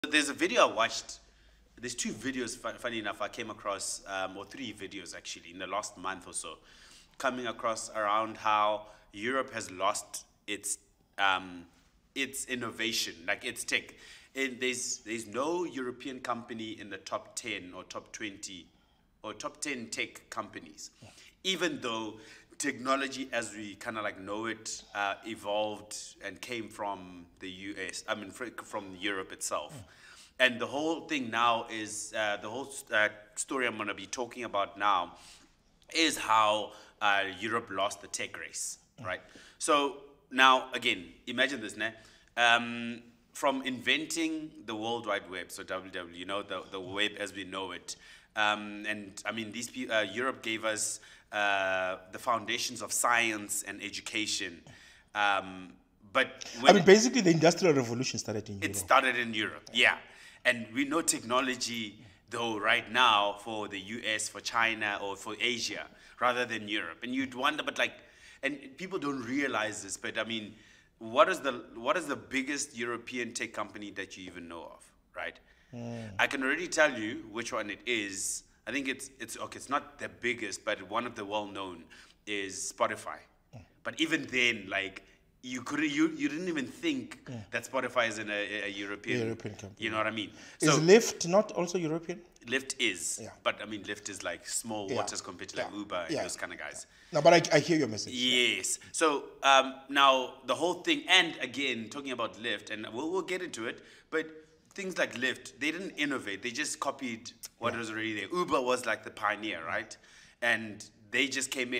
Is Europe Losing Its Tech Edge? 🤔💔
A riveting analysis unfolds as two insightful speakers explore Europe's surprising fall from grace in the tech race. Once the cradle of innovation—from the creation of the World Wide Web to the roots of the Industrial Revolution—Europe now finds itself overshadowed by tech giants in the US and Asia. Discover how European enterprises like Spotify and Lyft struggle for prominence against their American counterparts and the risks of merely replicating existing models rather than innovating. Explore the nuances of a continent rich in scientific history but facing challenges in the digital age. Fear not, the discussion goes beyond just Europe, touching on the ambitious yet elusive Project Titan from Apple. A thought-provoking journey into the core of technological evolution awaits.